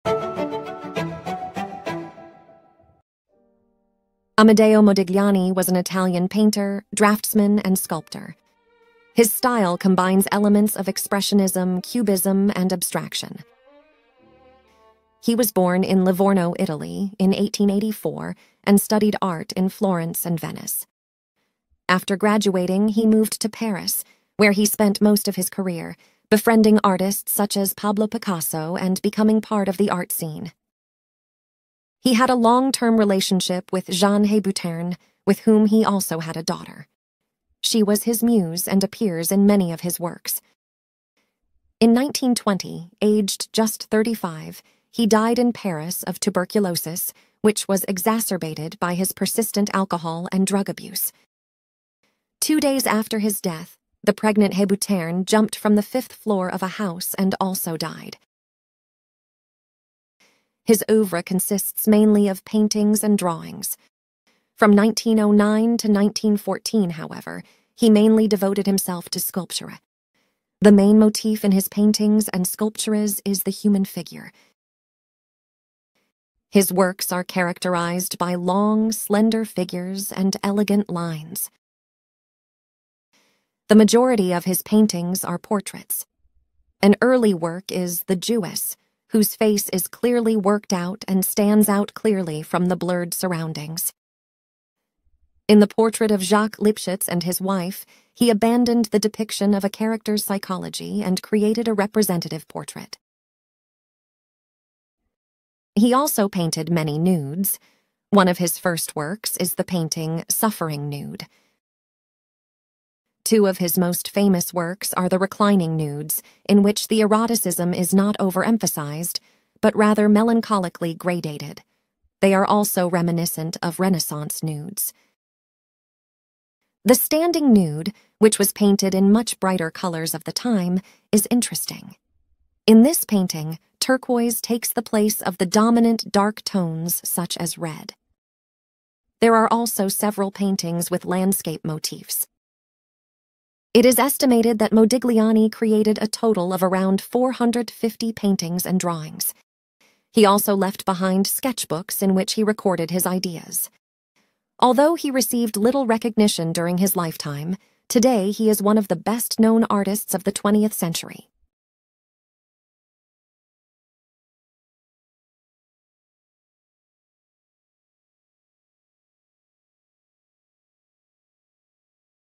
AMEDEO MODIGLIANI - Life, Works & Painting Style | Great Artists simply Explained in 3 minutes!
Amedeo Modigliani was an Italian painter, draughtsman and sculptor. His style combines elements of expressionism, cubism and abstraction.

He was born in Livorno, Italy, in 1884 and studied art in Florence and Venice. After graduating, he moved to Paris, where he spent most of his career, befriending artists such as Pablo Picasso and becoming part of the art scene.

He had a long-term relationship with Jeanne Hébuterne, with whom he also had a daughter. She was his muse and appears in many of his works. In 1920, aged just 35, he died in Paris of tuberculosis, which was exacerbated by his persistent alcohol and drug abuse. Two days after his death, the pregnant Hébuterne jumped from the fifth floor of a house and also died.

His oeuvre consists mainly of paintings and drawings. From 1909 to 1914, however, he mainly devoted himself to sculpture.  The main motif in his paintings and sculptures is the human figure. His works are characterized by long, slender figures and elegant lines.

The majority of his paintings are portraits. An early work is The Jewess, whose face is clearly worked out and stands out clearly from the blurred surroundings. In the portrait of Jacques Lipchitz and his Wife, he abandoned the depiction of a character's psychology and created a representative portrait.

He also painted many nudes. One of his first works is the painting Suffering Nude. Two of his most famous works are the Reclining Nudes, in which the eroticism is not overemphasized, but rather melancholically gradated. They are also reminiscent of Renaissance nudes. The standing nude, which was painted in much brighter colors of the time, is interesting.
In this painting, turquoise takes the place of the dominant dark tones such as red.

There are also several paintings with landscape motifs.
It is estimated that Modigliani created a total of around 450 paintings and drawings. He also left behind sketchbooks in which he recorded his ideas. Although he received little recognition during his lifetime, today he is one of the best-known artists of the 20th century.